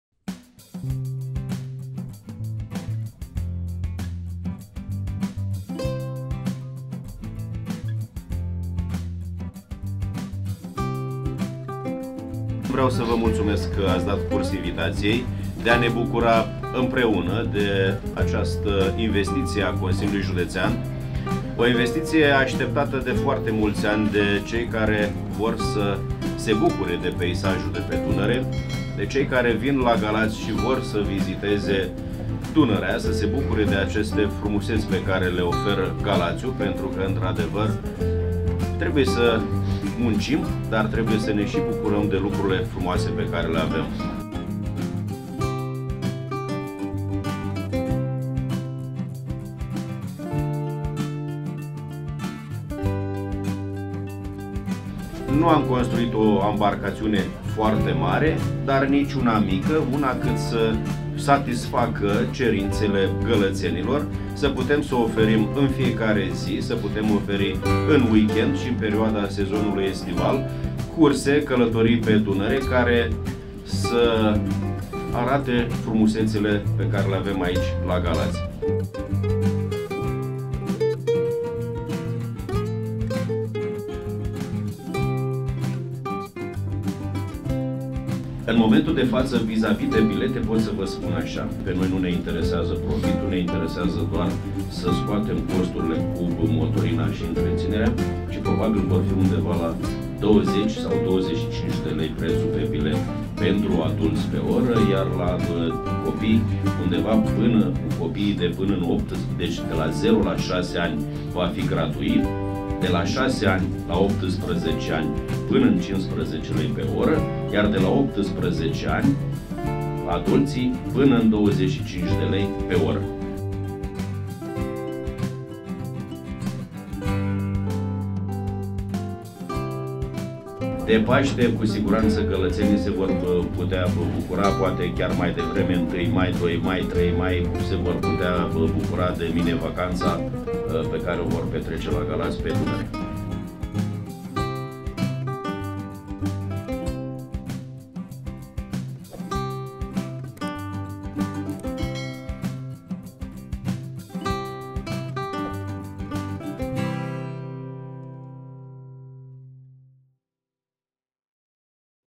Super! Navă de agrement pentru plimbări pe Dunăre, la Galați!
Videoreportaj realizat de: Ovidiu Amalinei